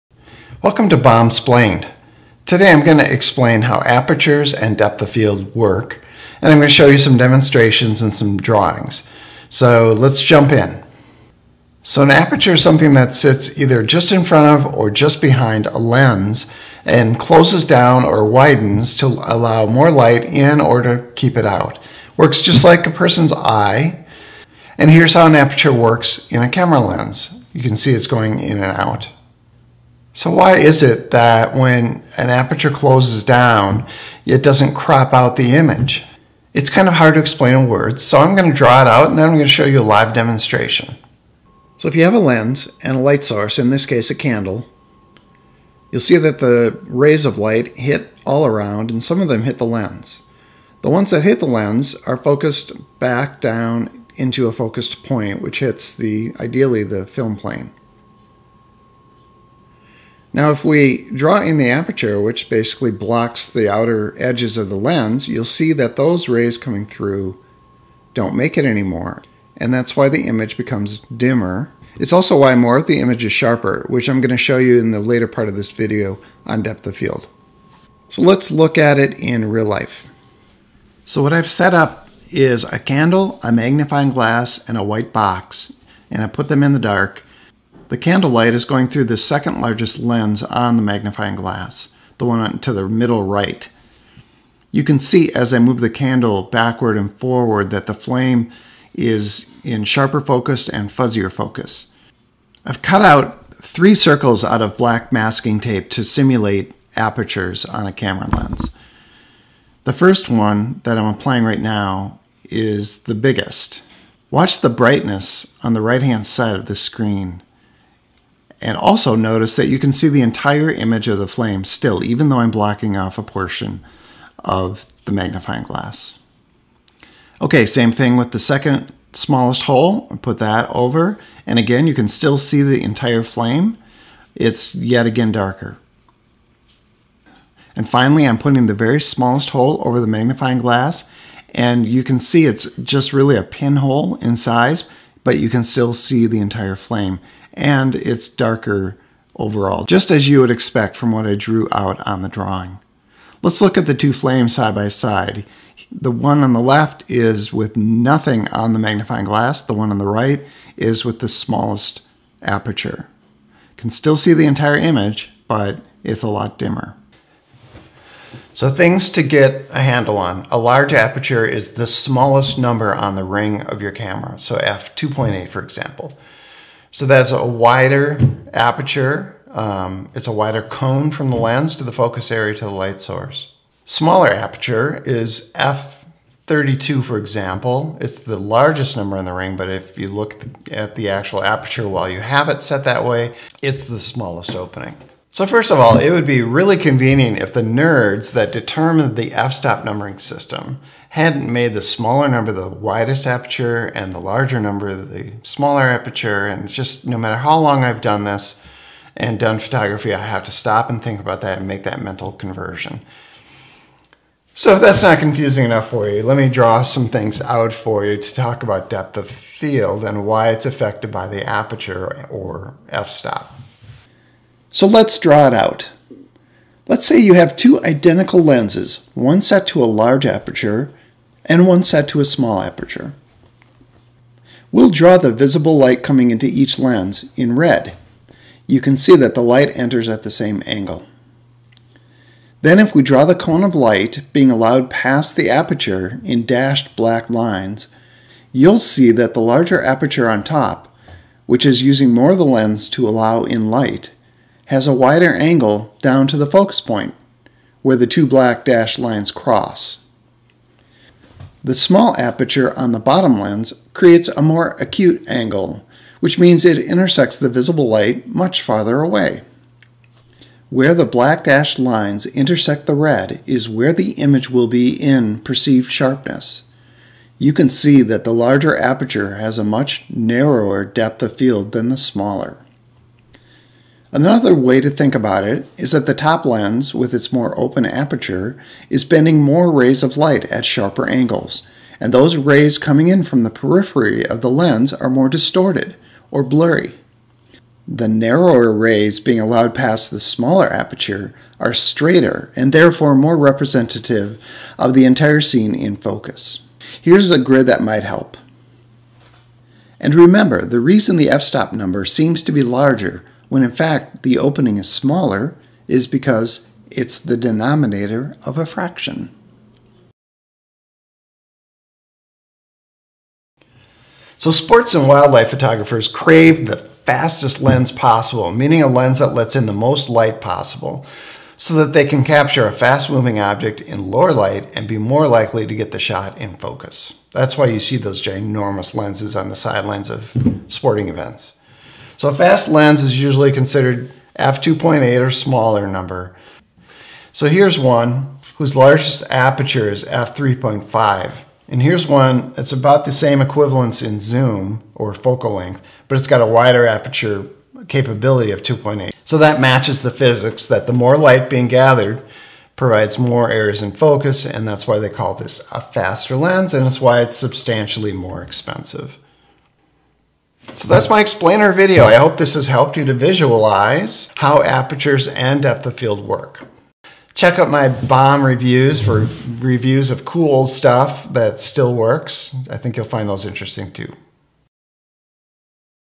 Simple Demo of How Apertures and Depth of Field Work
When you close the aperture on your camera lens, why is the image not cropped out?  And what does aperture have to do with how much is in focus in your picture?

In this explainer video, I demonstrate these concepts by using drawings and real life demonstrations.

You will also learn why a low aperture number on a lens equates to a wide open aperture, and vice versa (hint - they are fractions, and the number people usually use is the denominator of that fraction).

After watching this, you should be able to predict which aperture setting will get you the most pleasing bokeh (blurriness) in the background (another hint: it's the lower numbers).